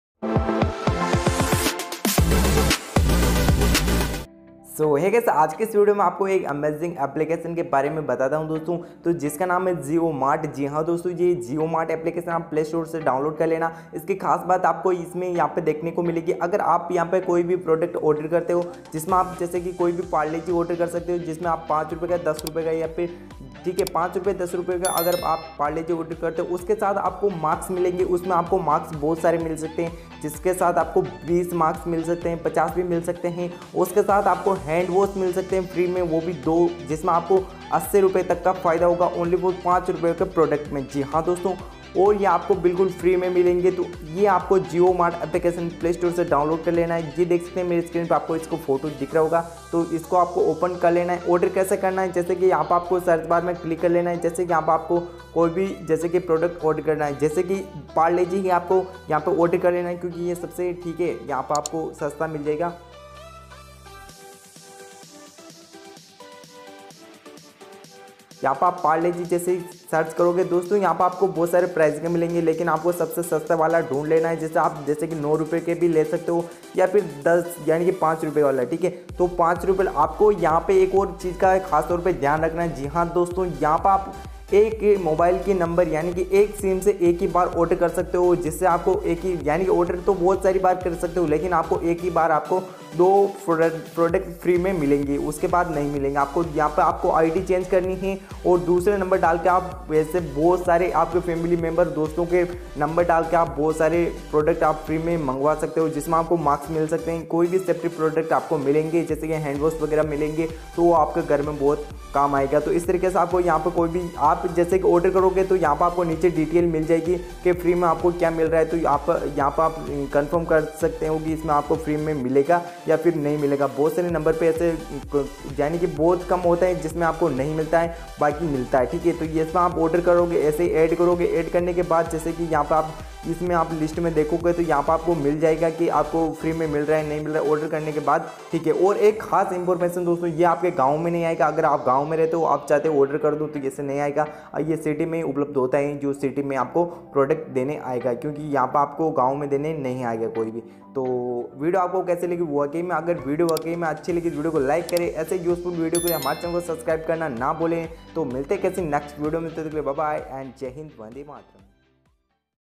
jio Mart shopping 🛍️ 🛒 || jio Mart dhamakedar offer jaldi se jao
jio mart shopping 🛒🛍️ || Jio mart dhamakedar offer jaldi se a jao

doston jiyo mat de raha hai bumper offer
5₹ ke Parle-G per 100₹ का product free जिओ मार्ट में मार्क्स सेनीटाइजर हैंड वॉश डेटोल हैंड वाश कोई भी प्रोडक्ट फ्री में मिल सकता है ₹5 के पारले जी पर तो जल्दी से जाओ और फ्री में लूट लो 😱💯 और हां एक नंबर पर एक ही बार मिलेगा ।

Jio Mart Shopping 🛒🛍️ bumper offer